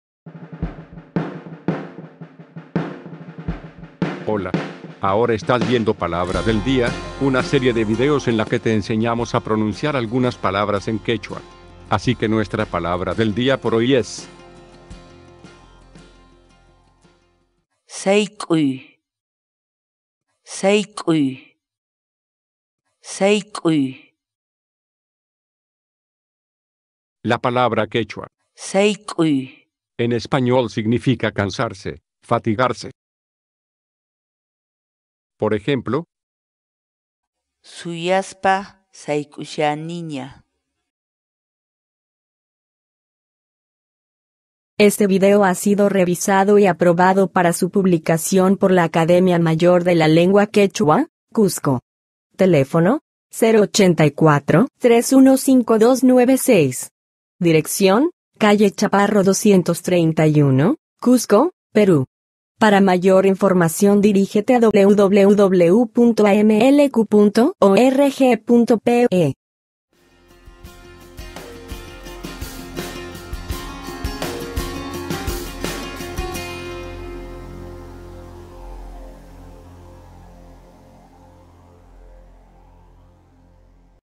Como pronunciar sayk'uy ‘cansarse, fatigarse’
Mira el video ahora en
Social

Grupo de Investigación Hinantin: La entrada ‘sayk'uy’ es parte de la base para el desarrollo de un diccionario hablado disponible gratuitamente para el idioma quechua.